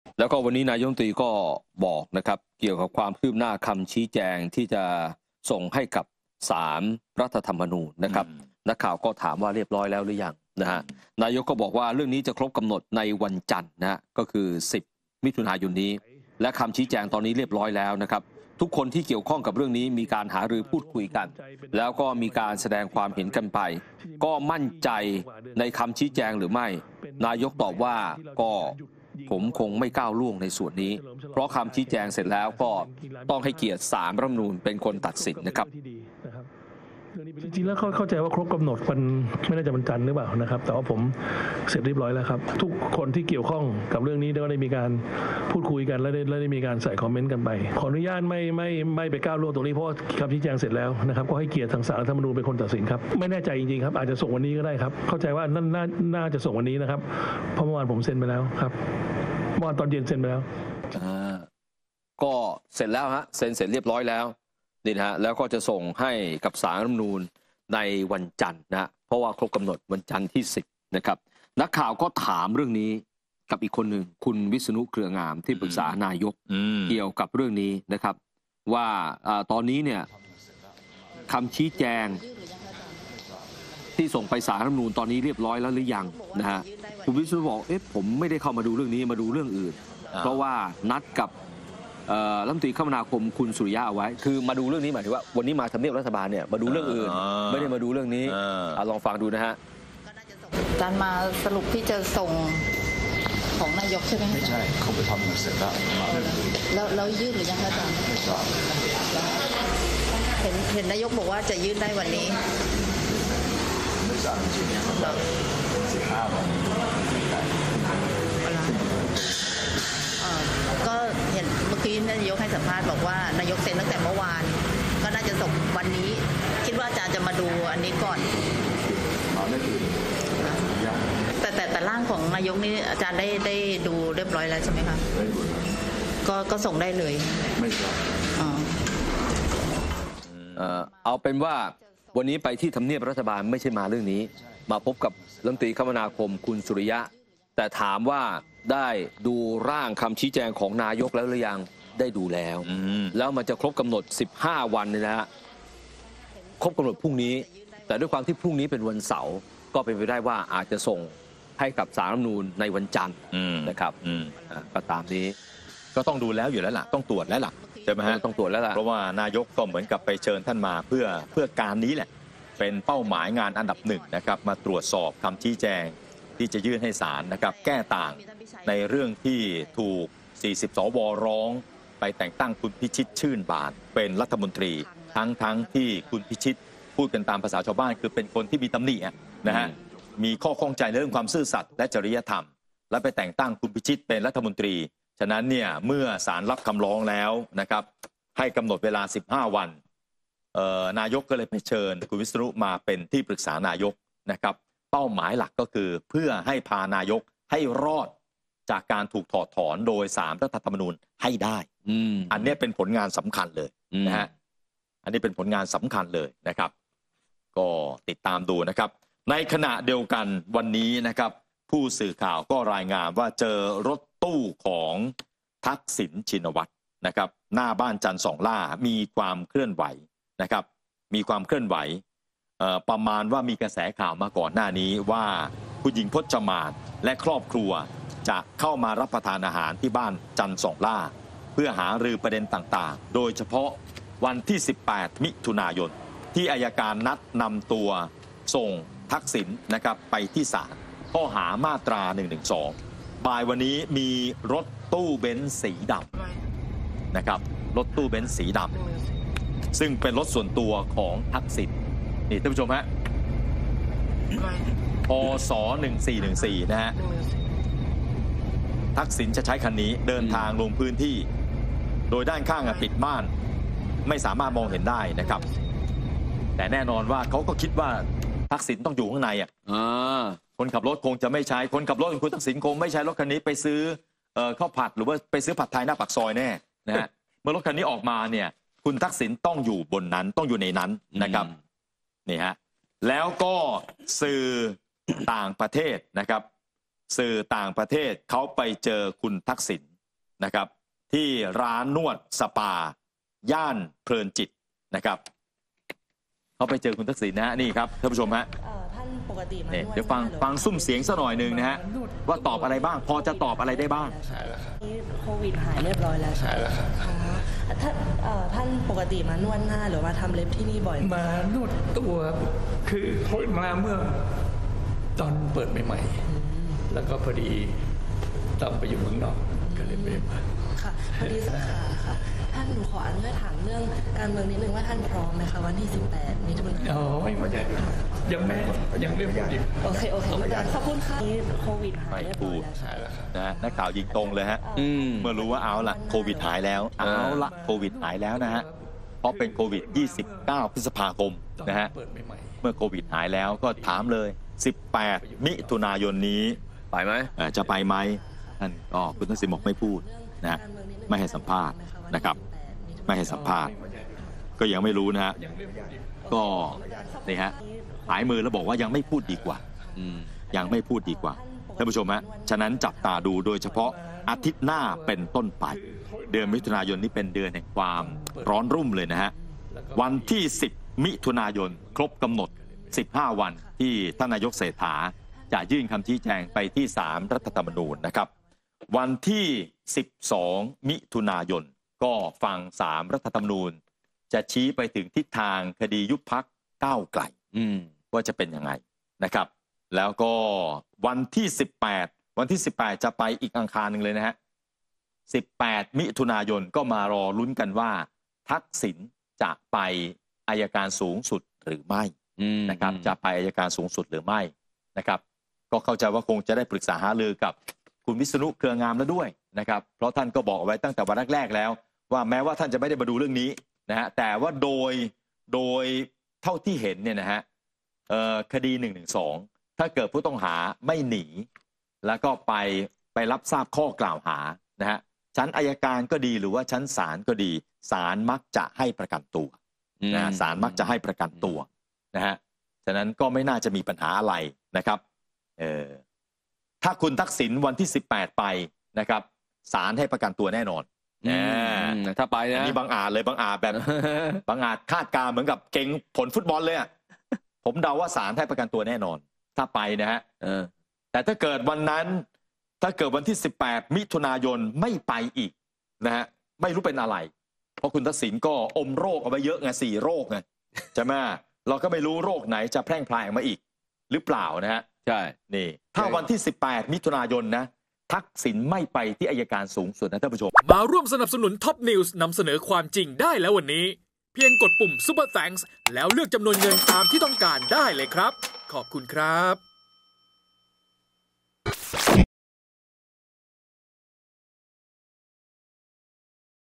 "กนก" พูดถึงปม "นายก" ถูก 40 สว. ร้องไปแต่งตั้ง "พิชิต" เป็น รมต.ทั้งที่เป็นคนมีตำหนิ | TOPNEWSTV
"กนก" พูดถึงปม "นายก" ถูก 40 สว. ร้องไปแต่งตั้ง "พิชิต" เป็น รมต. ทั้งที่เป็นคนมีตำหนิ "นายกฯ" เชิญ "วิษณุ" เป็นที่ปรึกษา เพื่อให้รอดจากการถูกถอดถอน